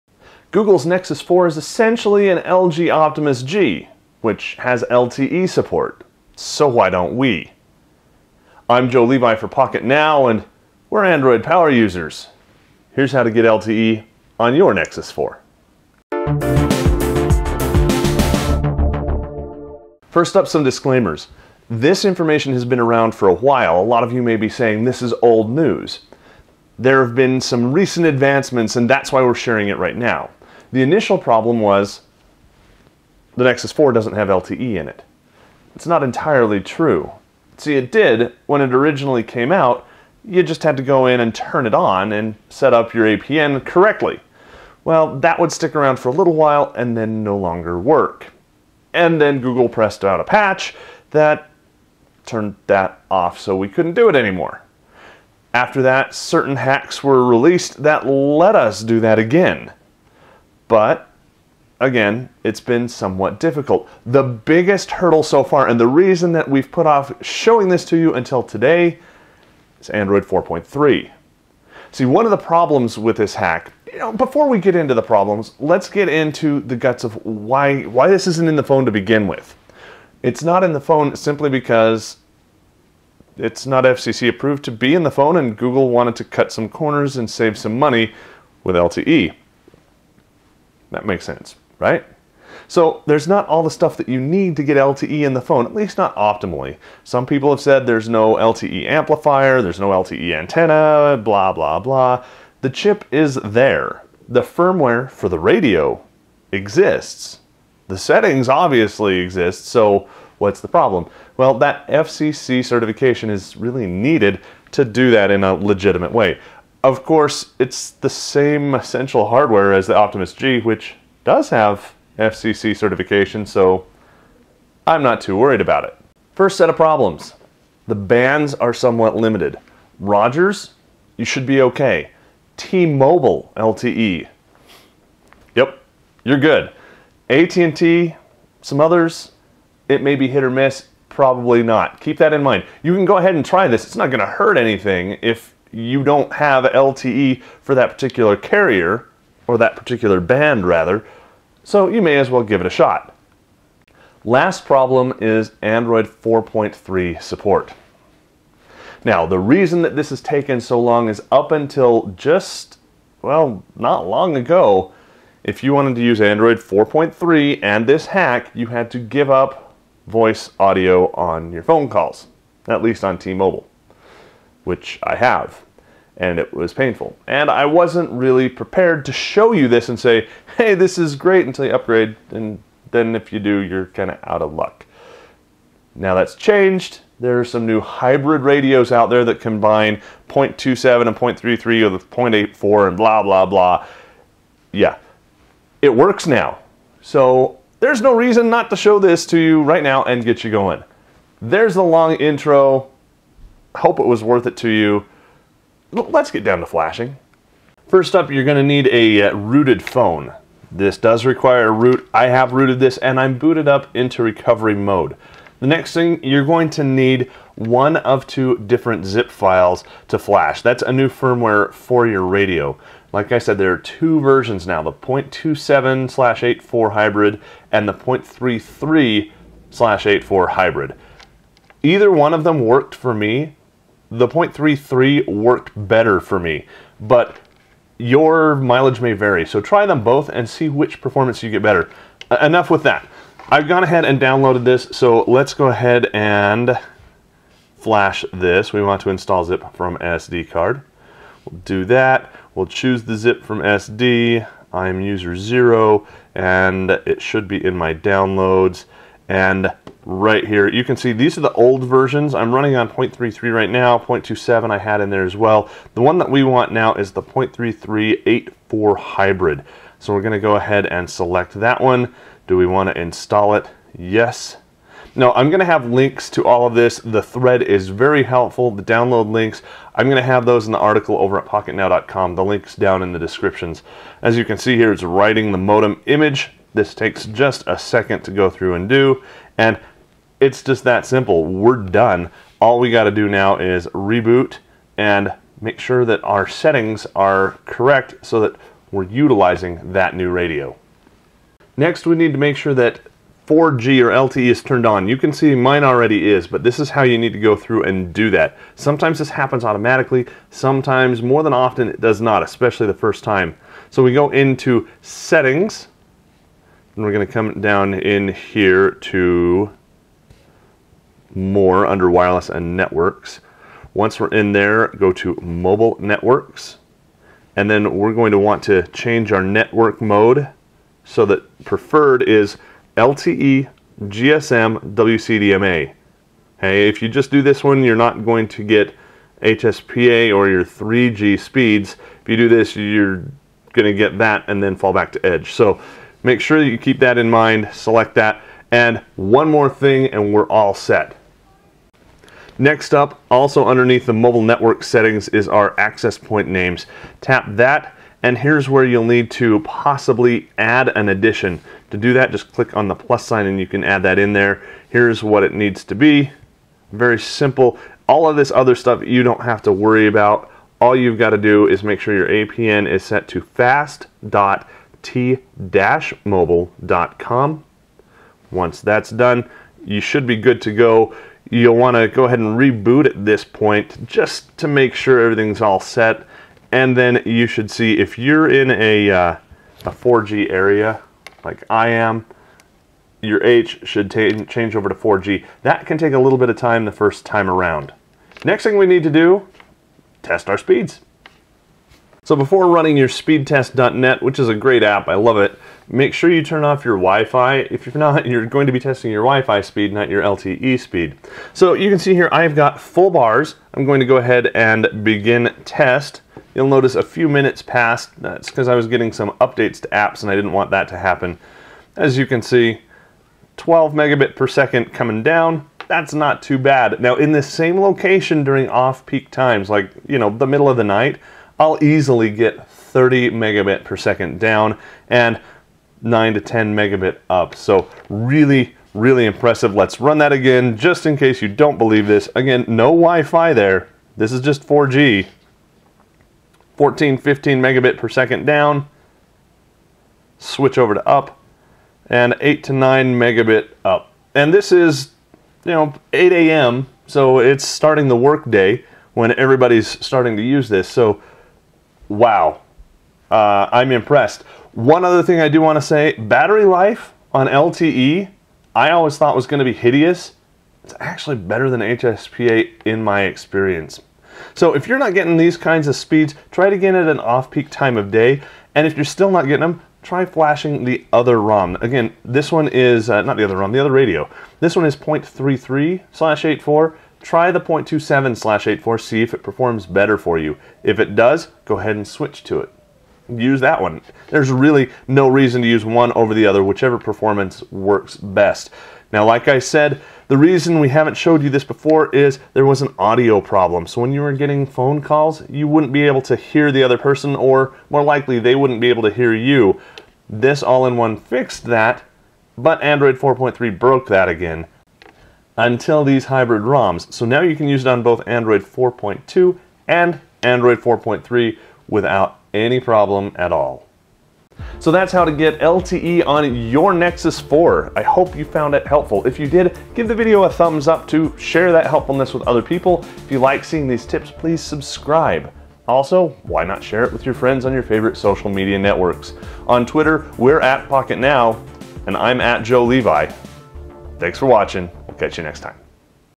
How to get LTE on your Nexus 4 | Pocketnow
Get the files here: /2013/08/02/get-lte-on-your-nexus-4

Google's Nexus 4 is essentially an LG Optimus G, which has LTE support -- so why don't we? We're Android Power Users, here's how to get LTE on your Nexus 4.